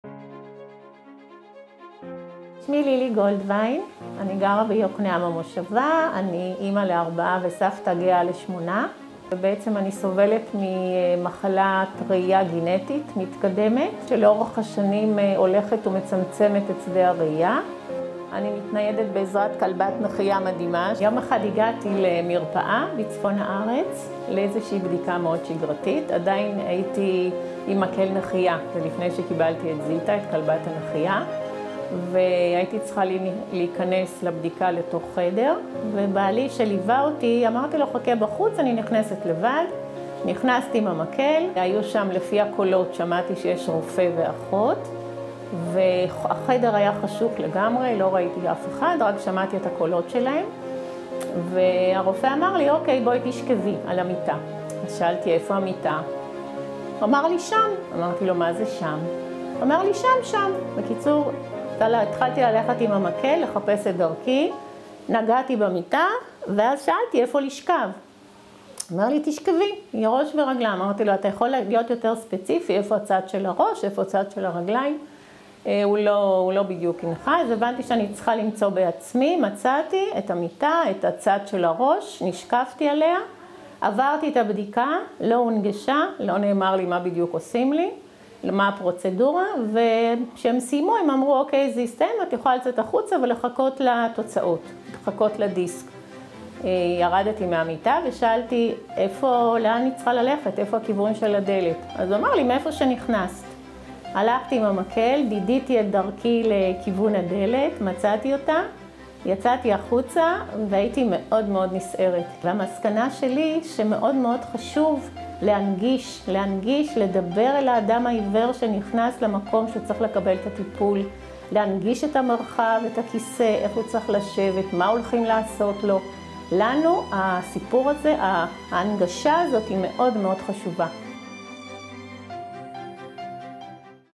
הסיפור של לילי
אנשים עם מוגבלות נתקלים לא פעם בקשיים בקבלת שרות במערכת הבריאות.
לכבוד יום שוויון לאנשים עם מוגבלות ,בחרנו ברמב"ם לתת לאנשים עצמם
לספר לנו מה חשוב להם בבואם לקבל שרות רפואי. הקשיבו להם !

נגשות=רגישות